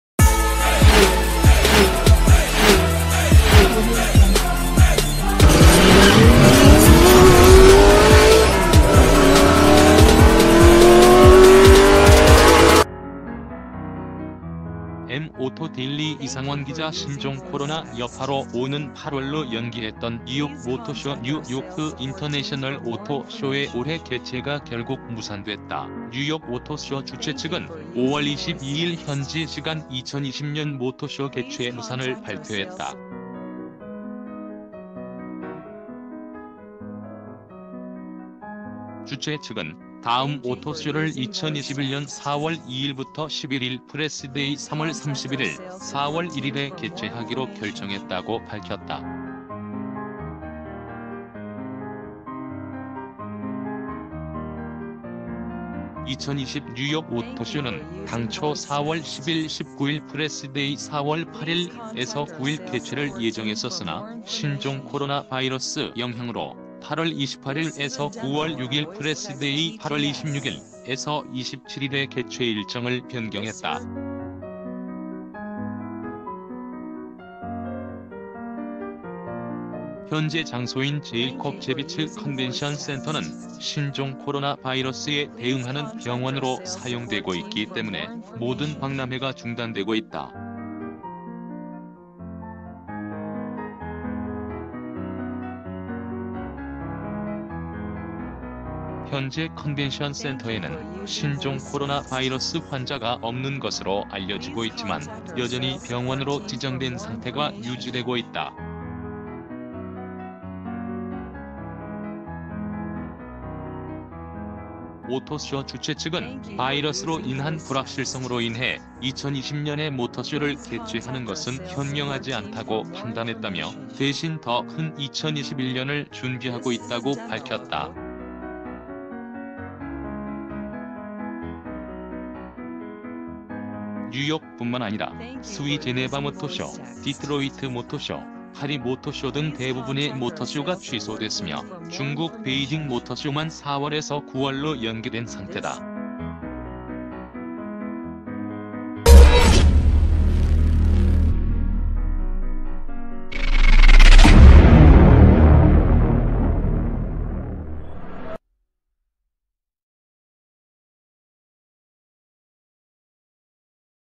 8월로 연기된 뉴욕 오토쇼, 올해 개최 결국 무산
8월로 연기된 뉴욕 오토쇼, 올해 개최 결국 무산 ►Subscribe for more ►Donation [M 오토데일리 이상원기자] 신종 코로나 여파로 오는 8월로 연기했던 뉴욕 모터쇼(New York International Auto Show)의 올해 개최가 결국